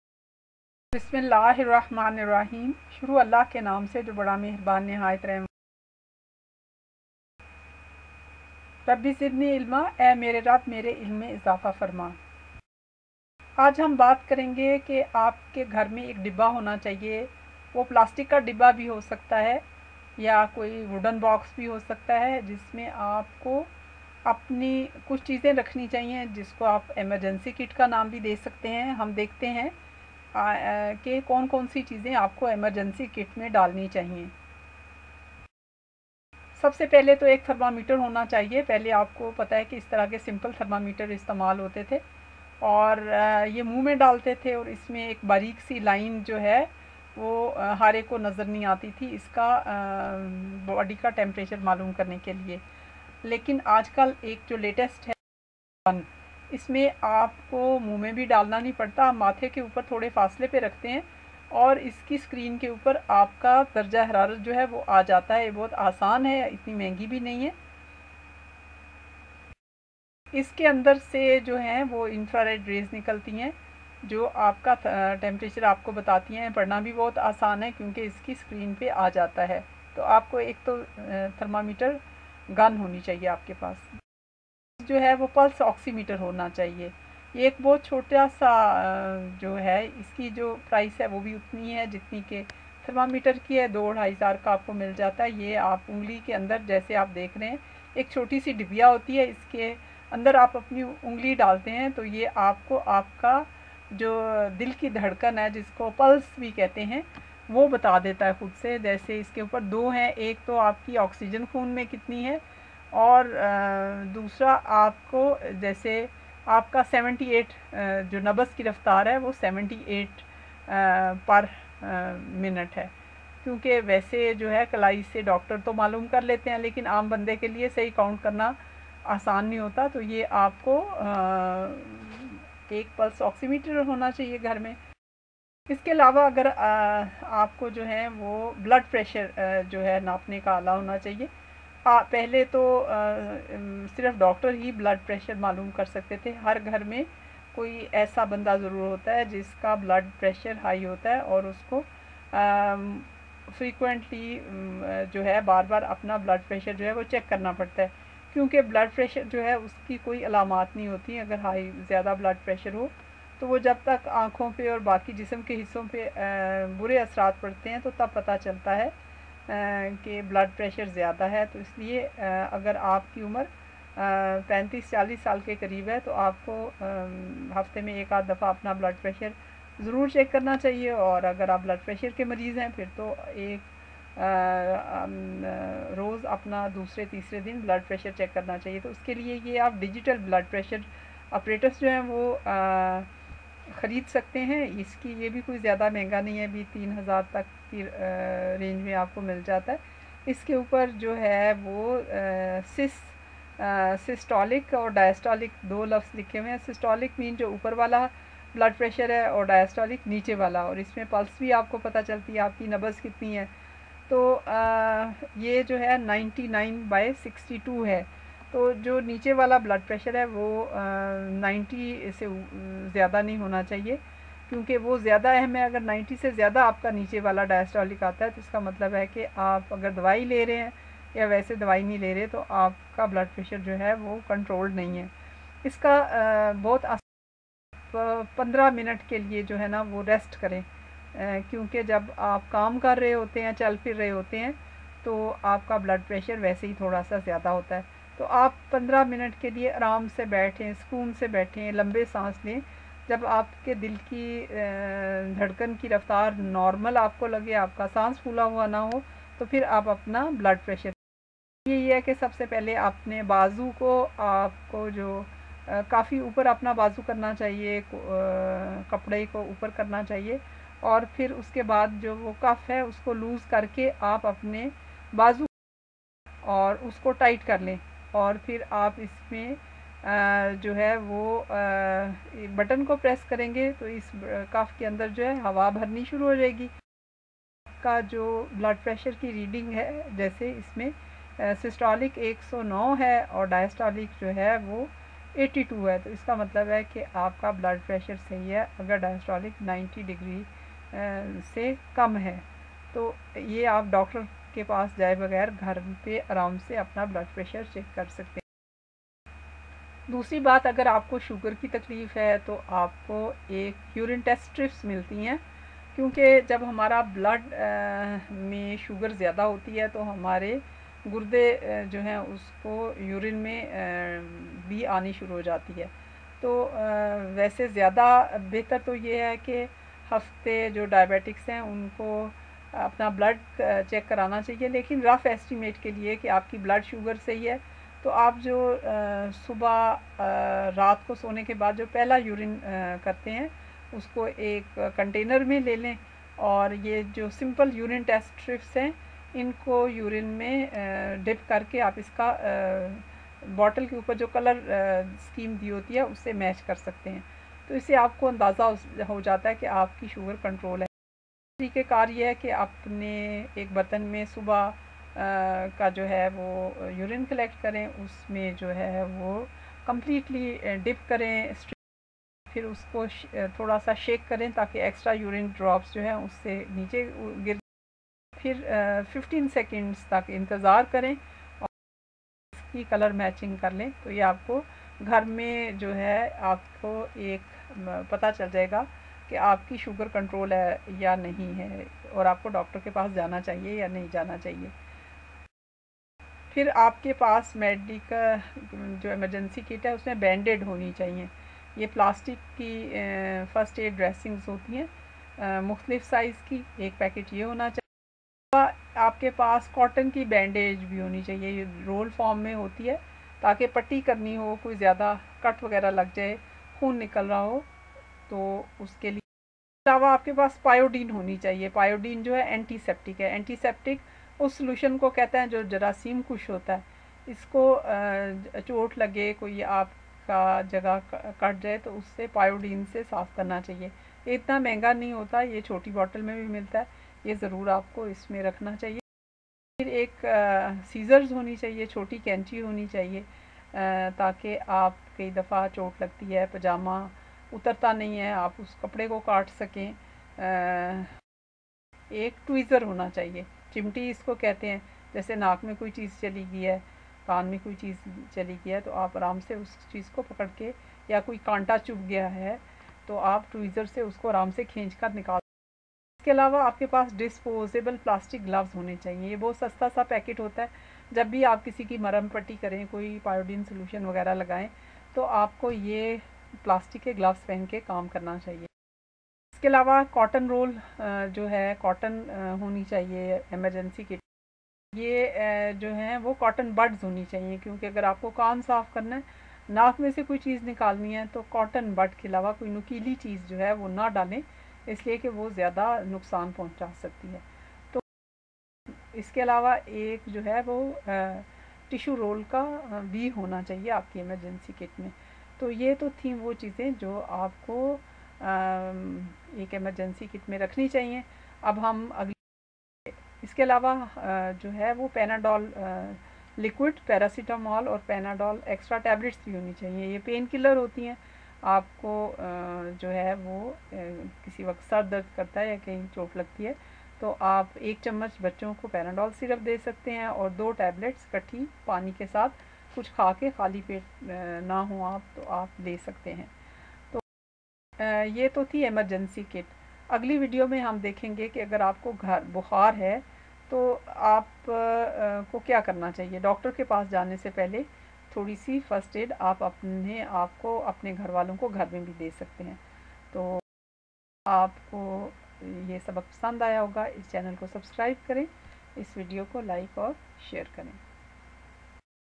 ایمرجنسی کٹ میں کیا ہونا چاہیے؟
ایمرجنسی کٹ میں کیا ہونا چاہیے؟ تھرما میٹر، بلڈ پریشر ماپنے کا آلہ قینچی، بینڈ ایڈ اور کاٹن رول وغیرہ
ہر پڑھے لکھے شخص کو ابتدایؑ طبی امداد کی باتیں معلوم ہونی چاہیں